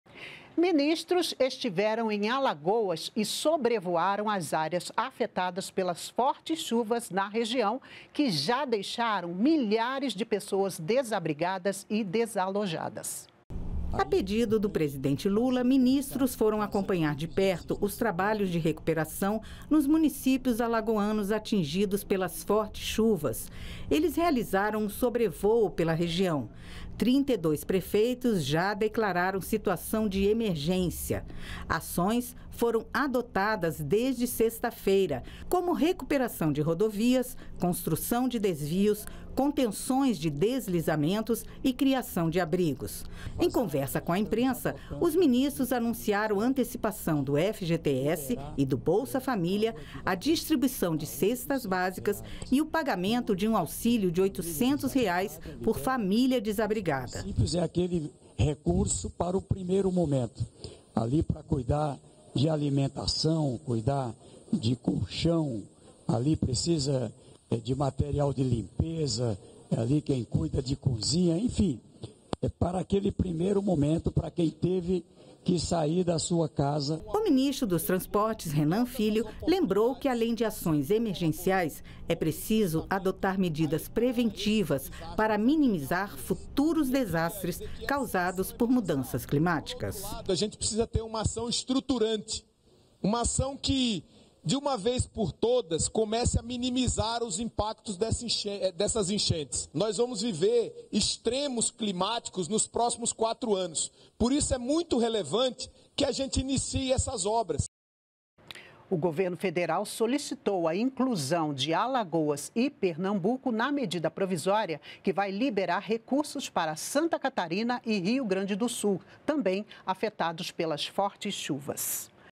CHUVAS EM ALAGOAS | FAMÍLIAS TERÃO AUXÍLIO DE R$ 800
12.07.23 -  Ministros estiveram em Alagoas e sobrevoaram as áreas afetadas pelas fortes chuvas na região, que já deixaram milhares de pessoas desabrigadas e desalojadas.